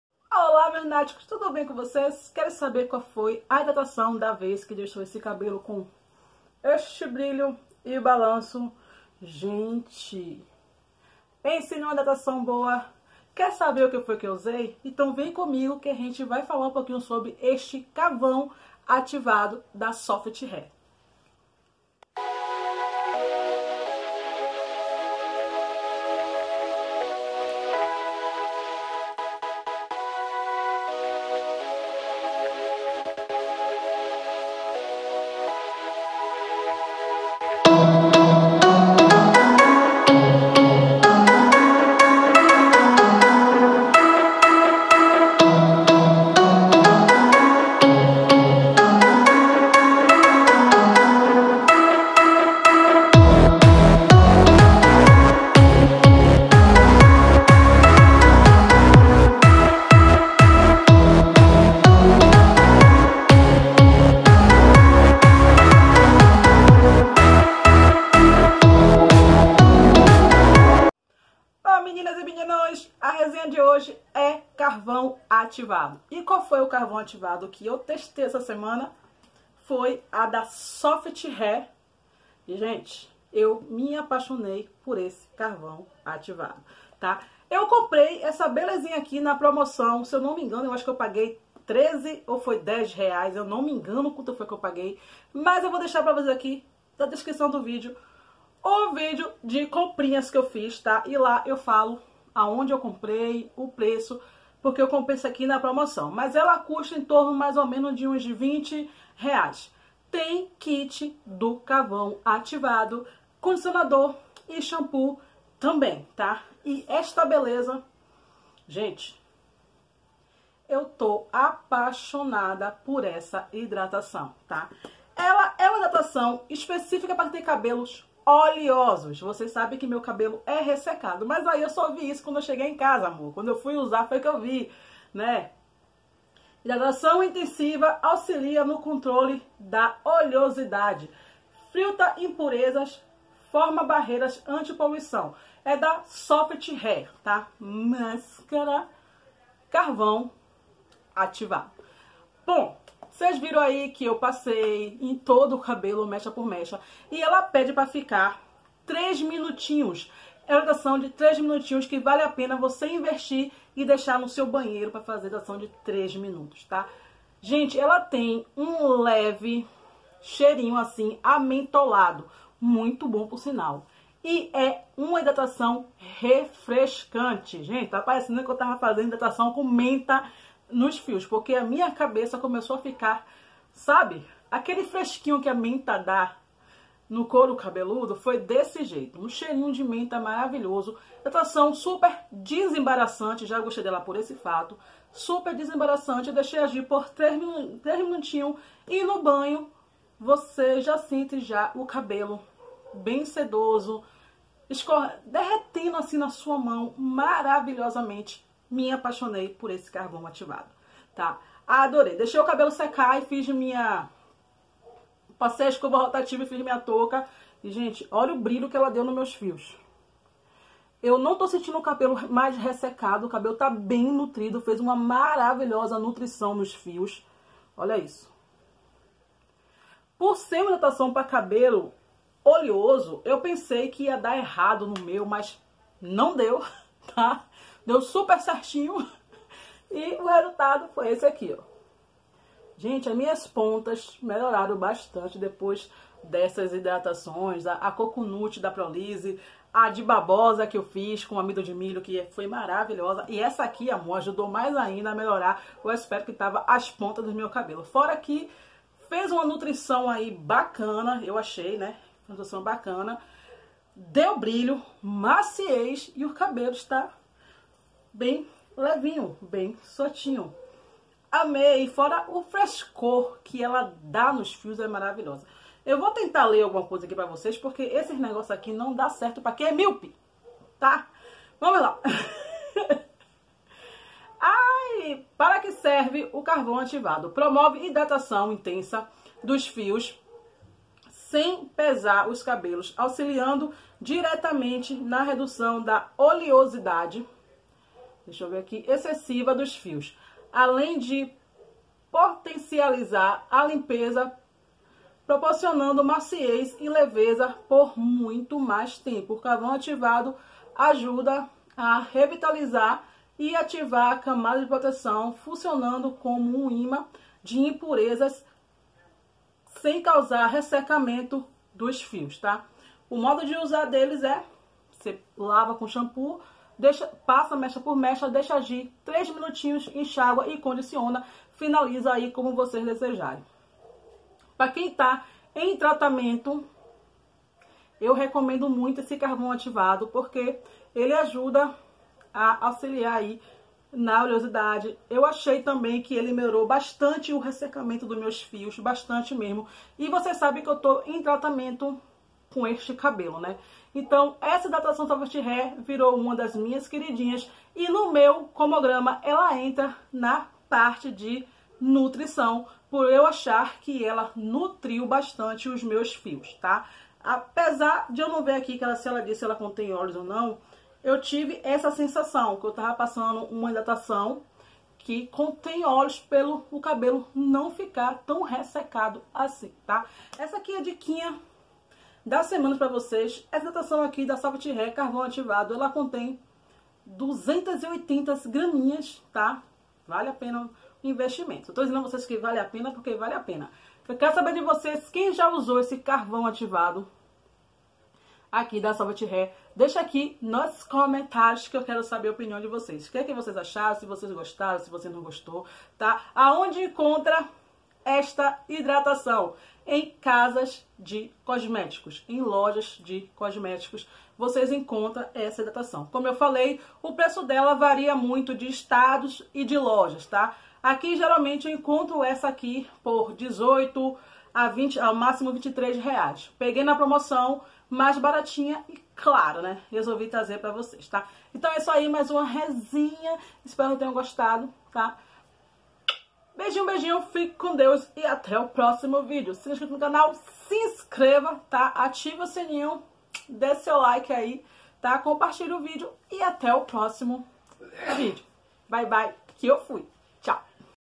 Carvão Ativado SoftHair I Hidratação Intensa
▬▬▬▬▬▬▬▬▬▬▬ஜ۩۞۩ஜ▬▬▬▬▬▬▬▬▬▬▬▬▬
Olá meus Lilynáticos, hj venho com uma hidratação maravilhosa, principalmente para aqueles que tem o cabelo oleoso, é uma boa pedida essa hidratação intensiva da SoftHair Carvão Ativado.